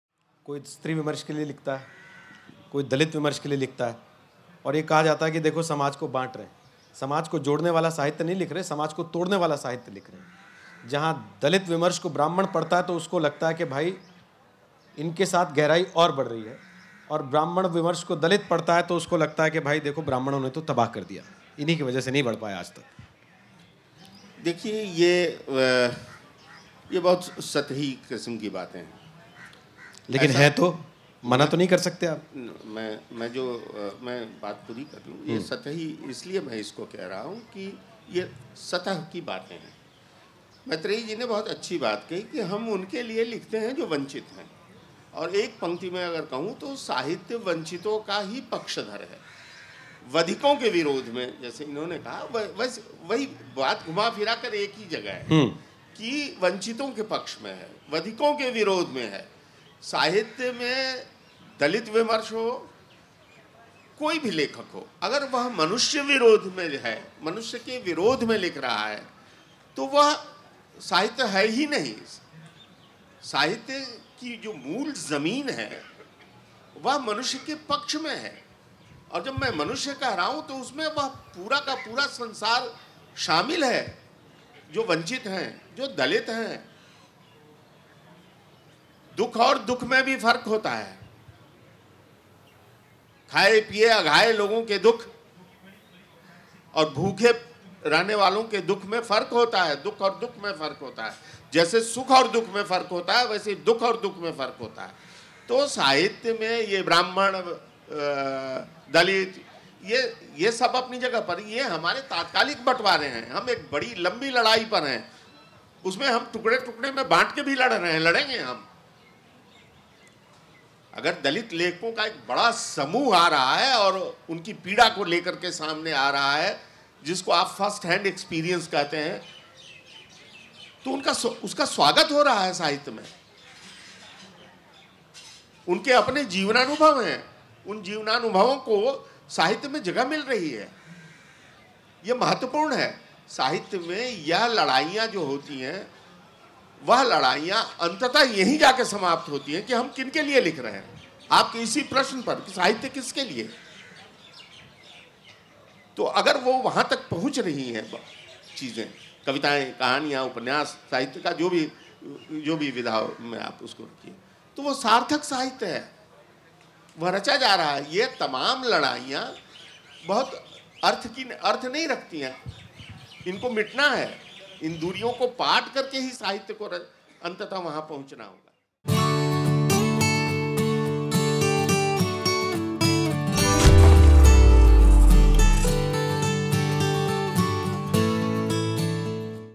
Hrishikesh Sulabh | दुख और दुख में फर्क होता है | Hindi Literature | Dalit Sahitya | Sahitya Tak
साहित्य की मूल जमीन मनुष्य के पक्ष में है. अगर मनुष्य के विरोध में कुछ लिखा जा रहा है तो वह साहित्य है ही नहीं. दुख और दुख में फर्क होता है जैसे दुख और सुख में फर्क होता है. साहित्य आजतक के मंच पर एंकर रोहित सरदाना के सवालों के जवाब में साहित्य में दलित, पिछड़े के बंटवारे पर लेखक ऋषिकेश सुलभ कहा कि अगर कोई दलितों की पीड़ा लेकर आ रहा है तो उसका स्वागत है. ये दूरियां सतह पर दिखाई दे रही हैं बाकी साहित्य एक है. ऋषिकेश सुलभ ने और क्या कहा, साहित्य तक पर सुनिए.